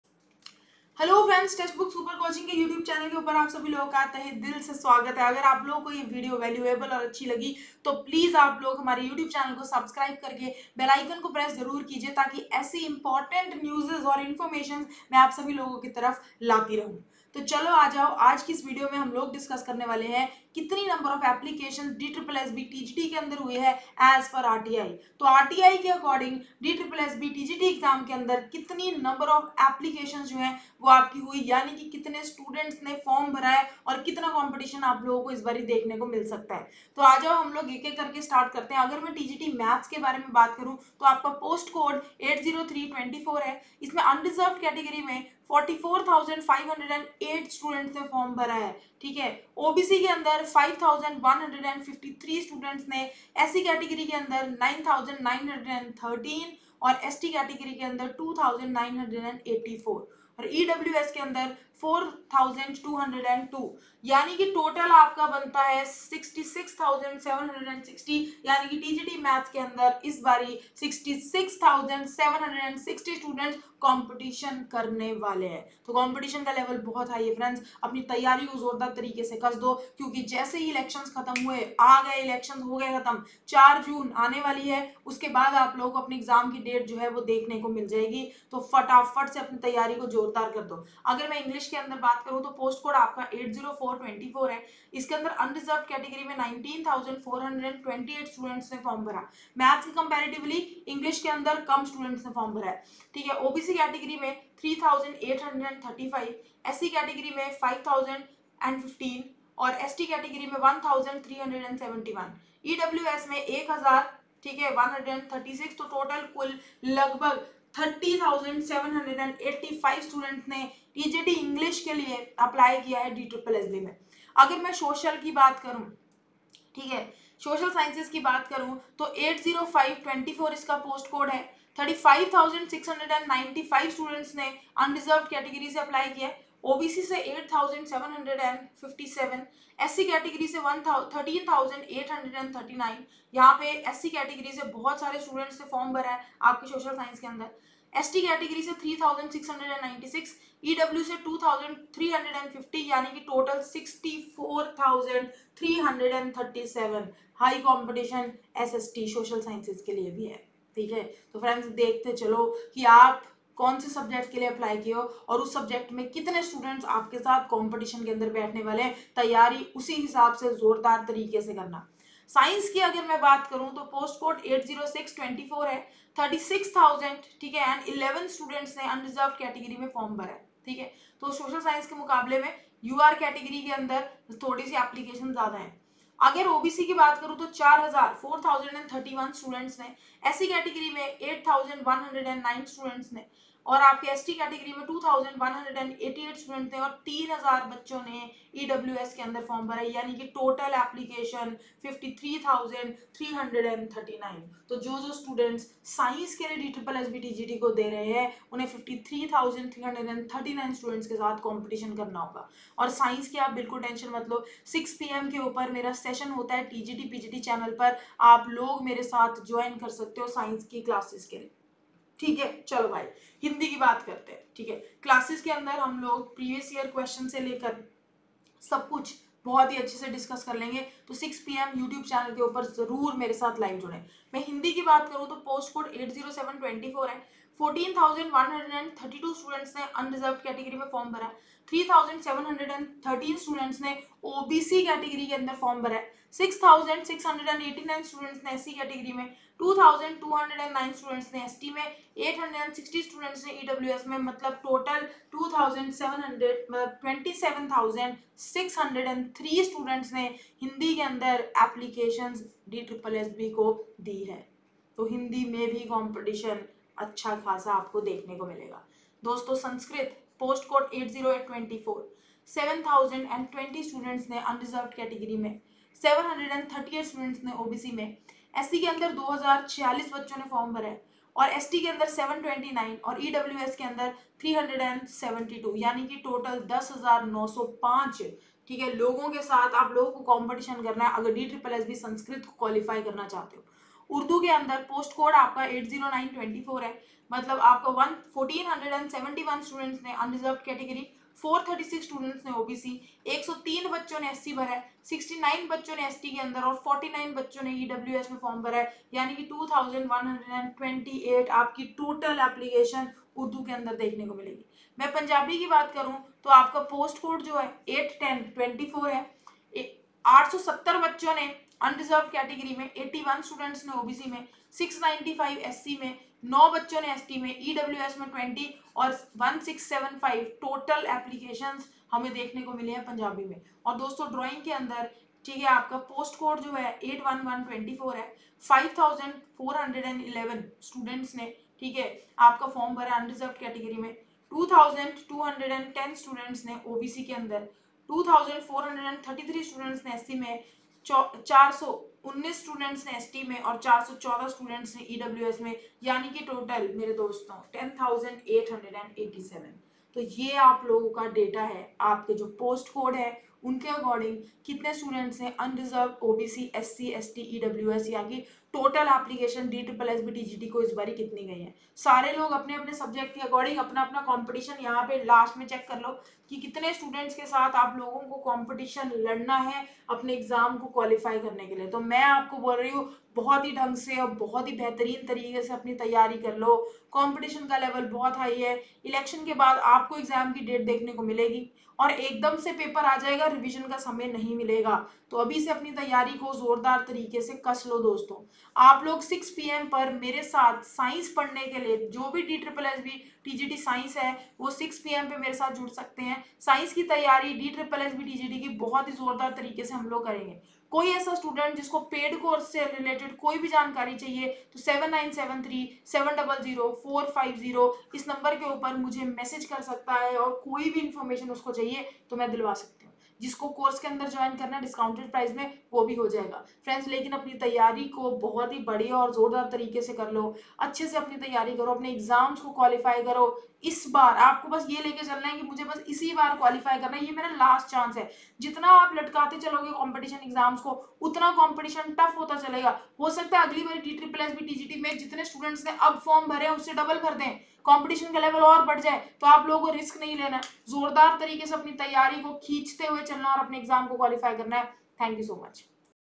DSSSB TGT Total Form Fill Up 2024 | DSSSB Exam Date 2024 | Ritika Ma'am
Welcome to our latest video on DSSSB TGT Total Form Fill Up 2024! In this video, Ritika Ma'am, our best faculty at Testbook, will guide you through the entire process of filling out the DSSSB TGT (Trained Graduate Teacher) application form for 2024.

In this session, you will learn step-by-step instructions on how to complete the DSSSB TGT 2024 form correctly. Ritika Ma'am will provide clear explanations and demonstrate each part of the form so you can avoid common mistakes. She will also share important tips and the essential documents required for the DSSSB TGT application process.

Additionally, we will discuss the DSSSB Exam Date 2024 and other key dates you need to mark on your calendar. Understanding these timelines will help you prepare better and stay ahead in your exam preparation journey.

Whether you are applying for the first time or need a refresher, this video is perfect for you. Make sure to watch till the end for some valuable tips and tricks.

🌟Use RITIKA code to get a discount on Testbook SuperCoaching.